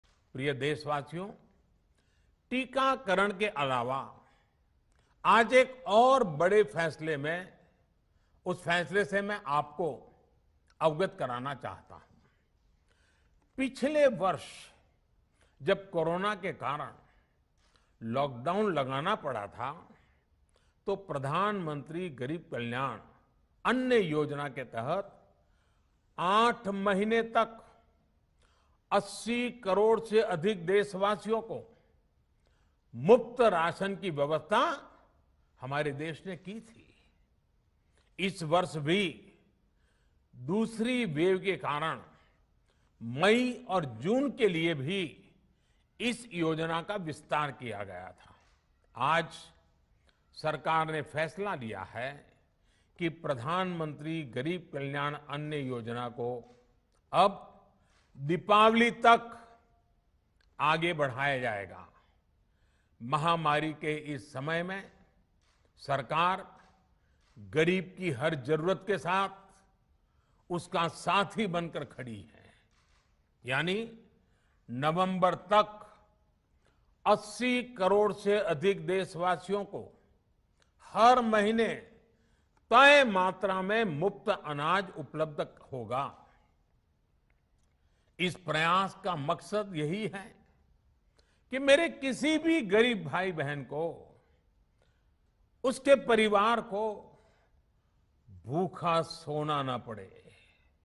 Centre extends Garib Kalyan Anna Yojana till Diwali, over 80 crore poor to get free ration
Prime Minister Narendra Modi addressed the nation on Covid-19 situation and vaccination drive. He said that the Centre has decided to once again take charge of procuring vaccines and will provide them to the States free of cost. Those in the 18-44 age group will get their doses free starting 21st June. PM Modi also announced the distribution of free rations under PM Garib Kalyan Yojana will be extended till Diwali.